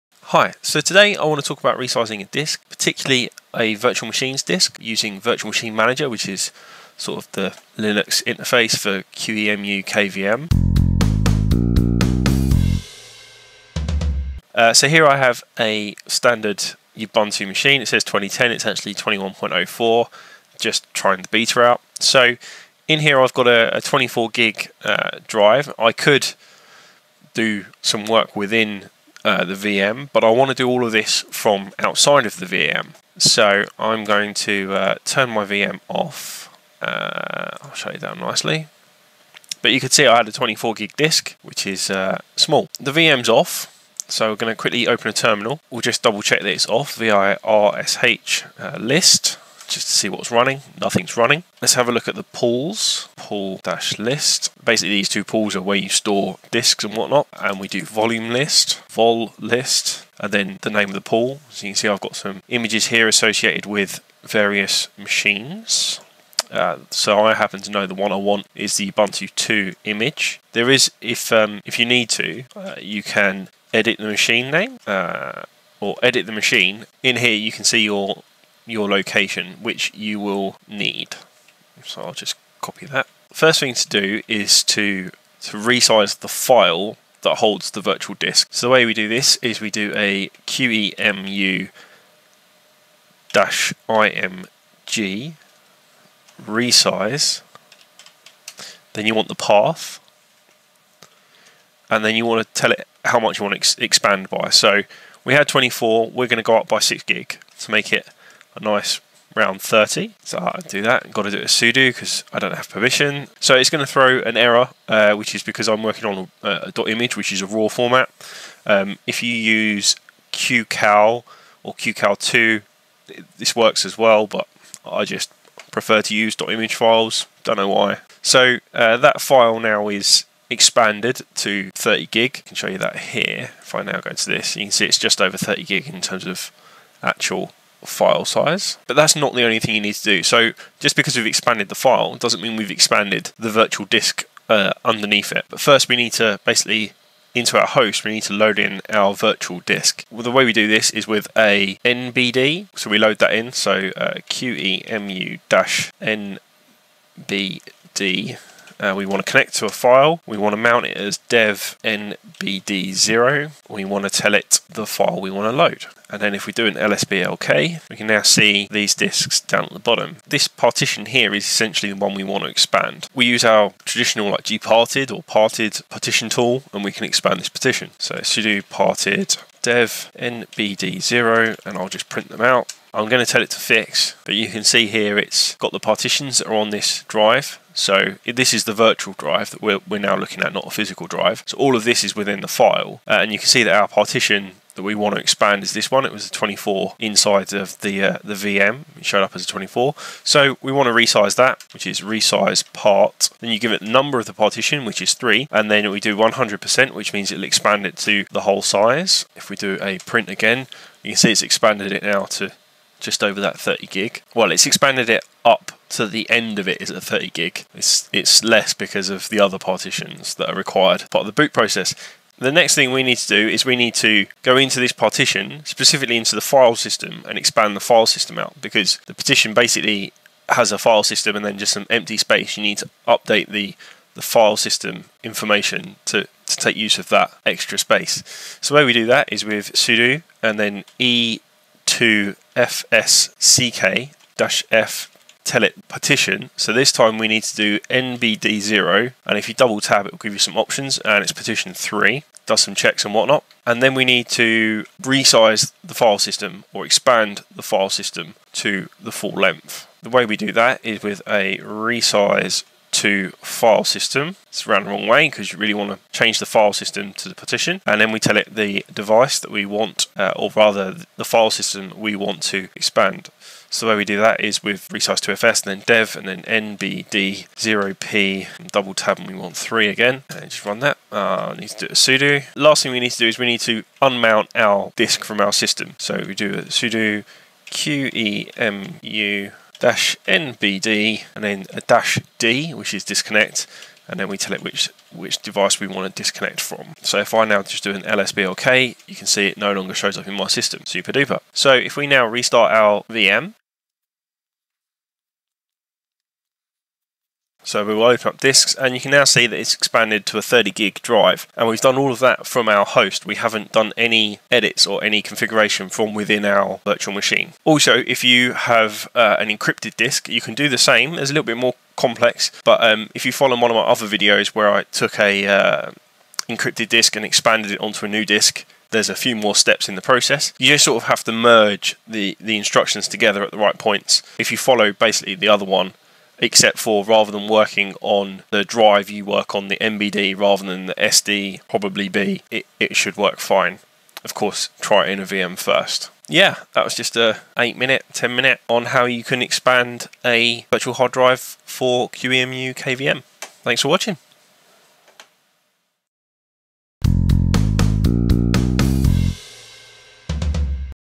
#60 - How to Expand a QEMU/KVM virtual disk from the host
Here i show how to expand a virtual machine's disk from the Host without any instruction needed with the guest machine - assuming linux guest with an ext filesystem.